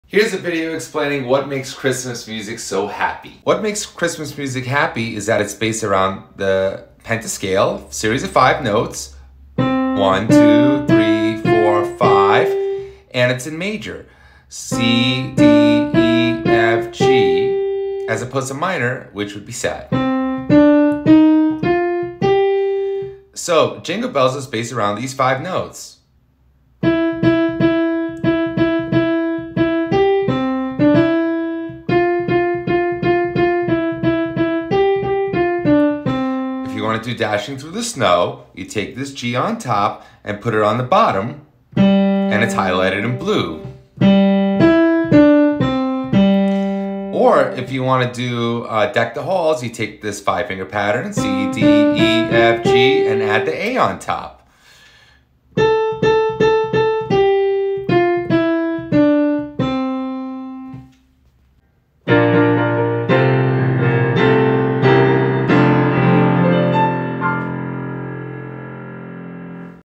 What Makes Christmas Music So Happy - A Playthrough
Explaining what makes Xmas music Happy, Simple, and Digestible!